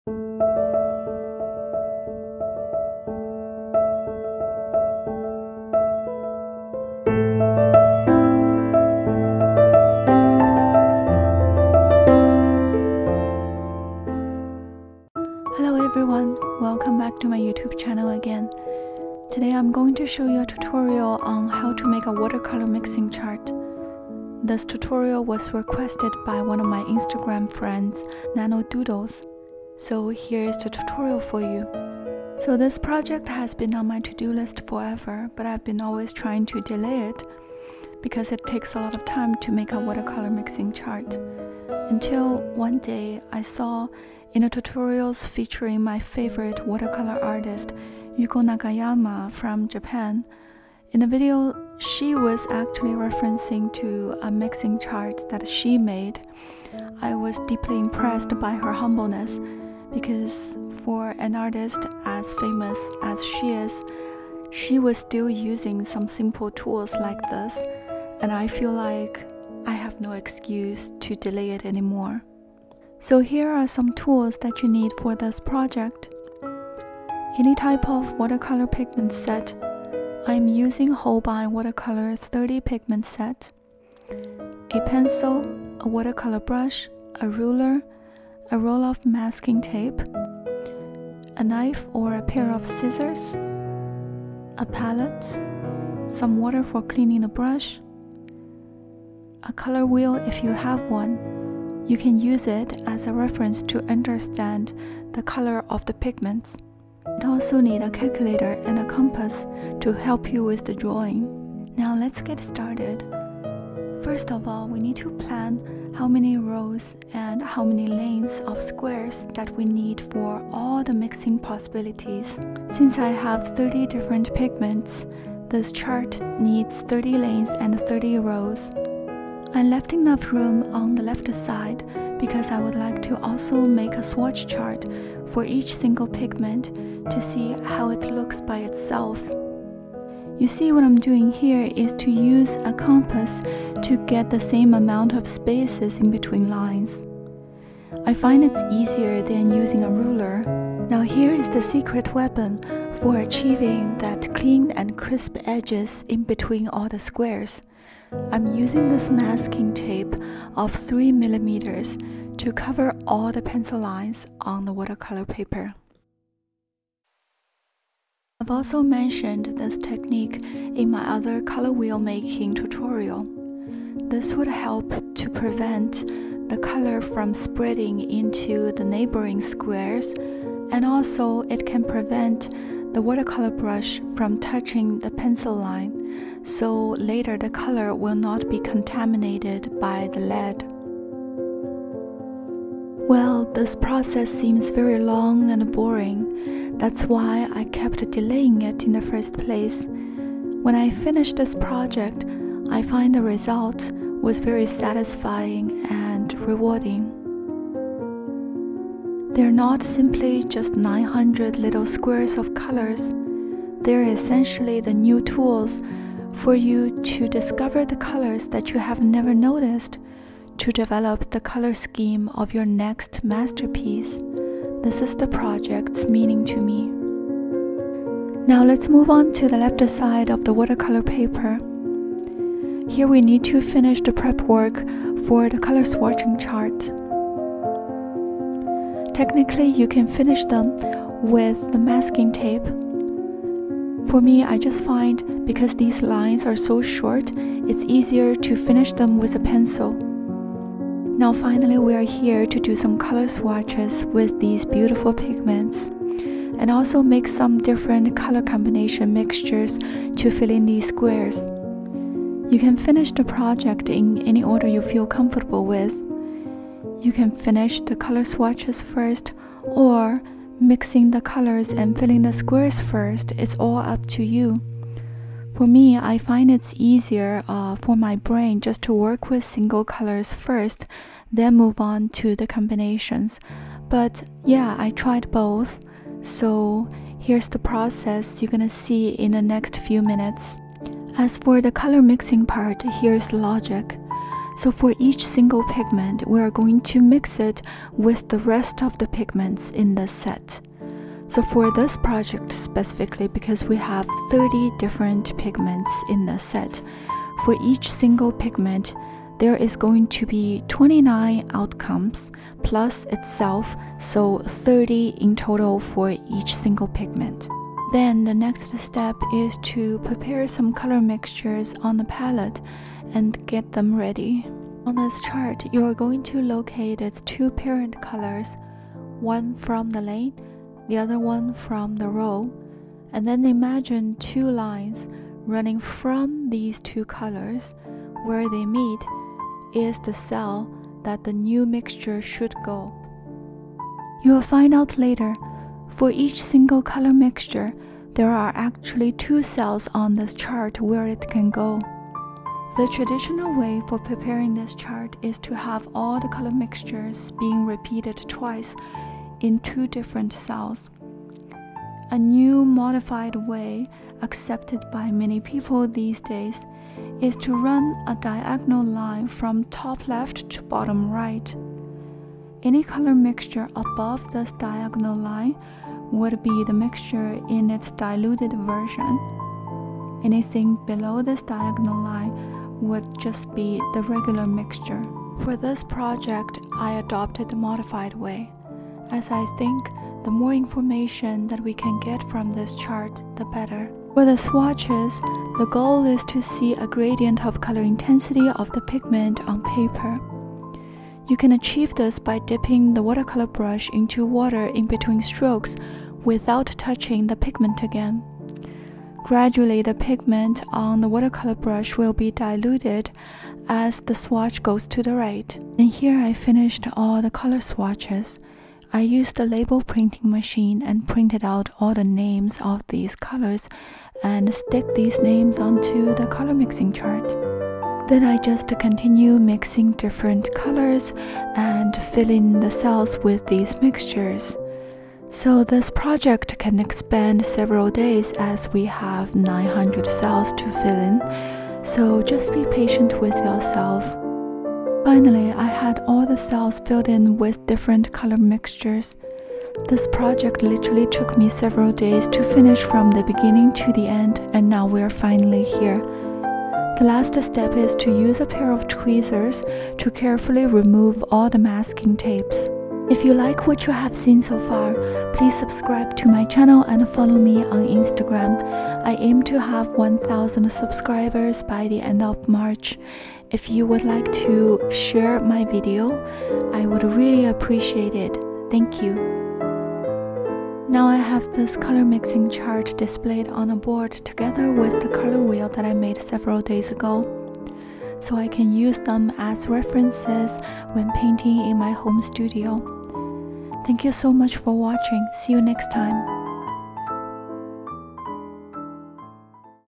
How to Make a Color Mixing Chart with Clean Crisp Edges with my Holbein Watercolor 30 Set - Tutorial
I finally made myself a watercolor color mixing chart and I would love to share the process with you. I just starting uploading more art tutorials and speed paintings again recently. Thanks!!!

Art | Creativity | Piano | Music | Life

Webpage: under construction...